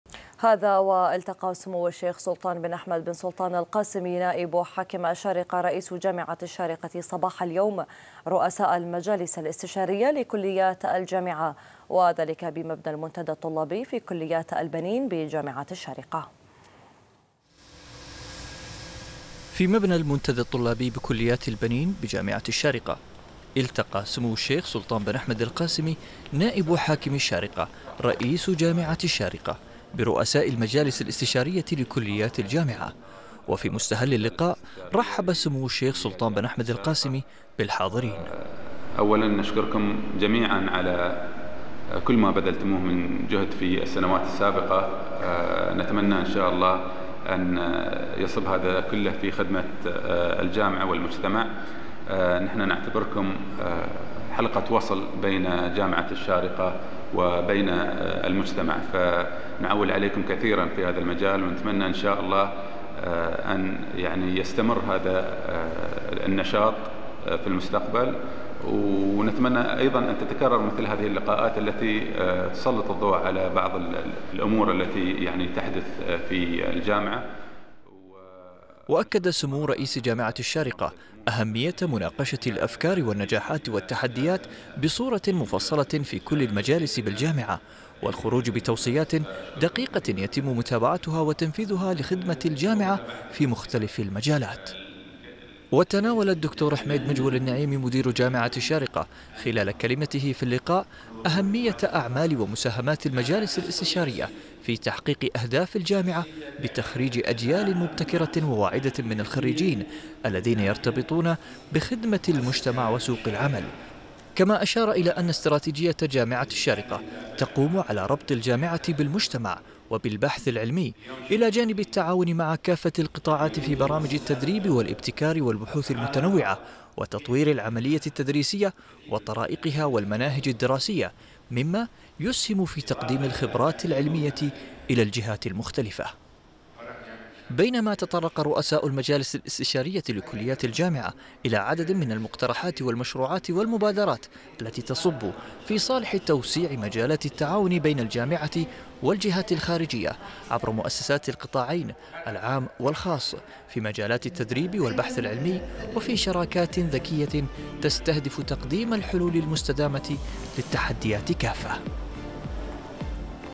أخبار الدار | سلطان بن أحمد القاسمي يلتقي رؤساء المجالس الاستشارية لكليات جامعة الشارقة
في مبنى المنتدى الطلابي بكليات البنين